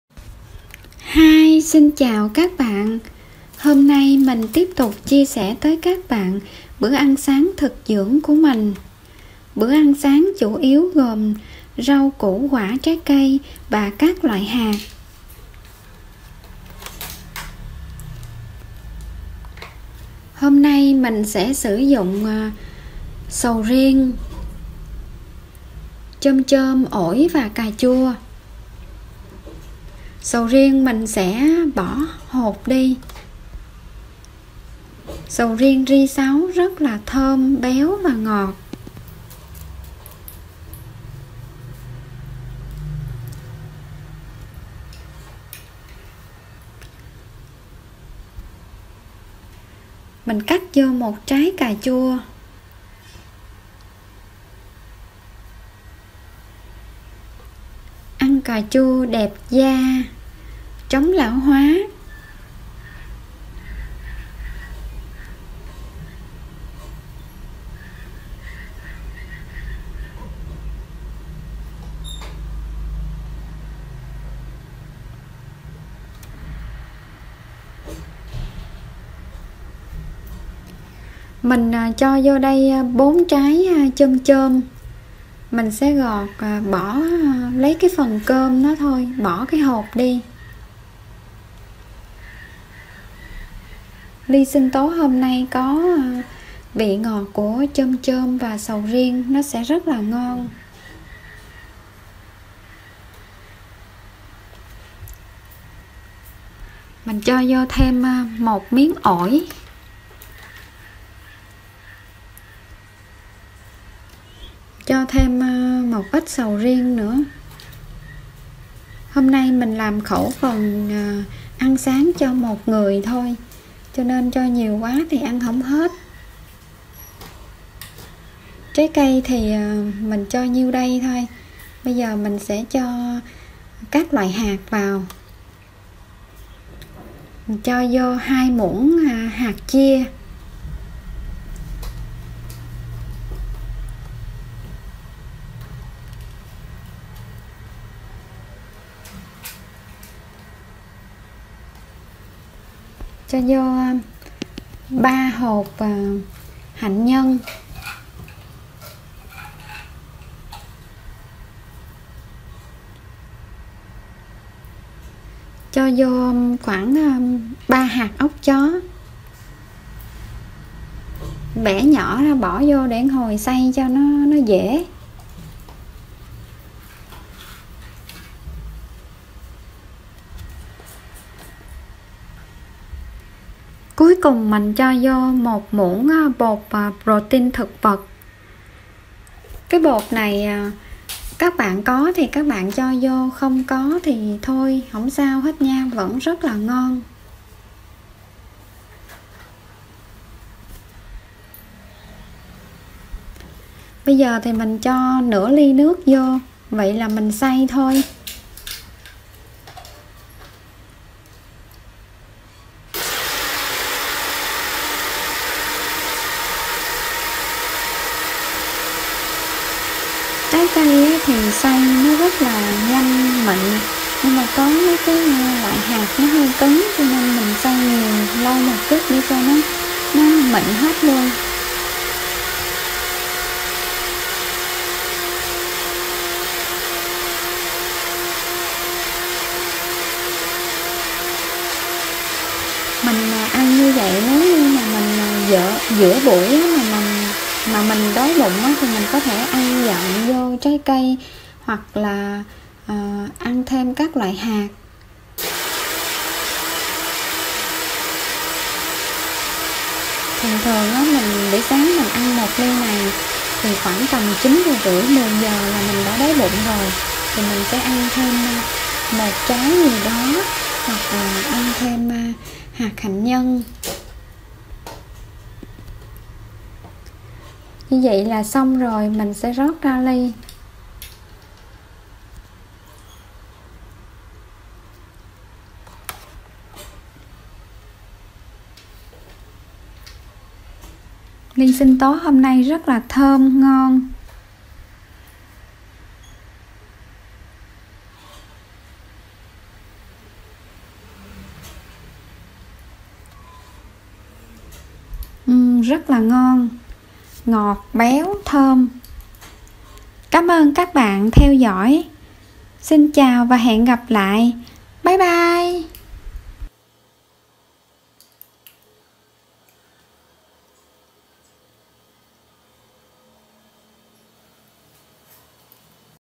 Thực Dưỡng| Bữa Sáng Dinh Dưỡng: Trái Cây Và Các Loại Hạt
Bữa Sáng Dinh Dưỡng: Trái Cây Và Các Loại Hạt
Nguyên liệu:
- Trái cây: Sầu riêng, chôm chôm, ổi, cà chua...
- Hạt: hạt hạnh nhân, hạt óc chó, hạt chia...
- Bột protein thực vật (nếu có)
Bữa ăn sáng rất quan trọng cho cơ thể, nó cung cấp năng lượng, dinh dưỡng cho cơ thể hoạt động nguyên ngày. Nếu bữa ăn tốt và đầy đủ dinh dưỡng thì nó giúp cơ thể tự chữa lành những tổn thương.
Chúc các bạn nhiều sức khỏe và bình an qua đại dịch này.
Cám ơn các bạn đã theo dõi. Love!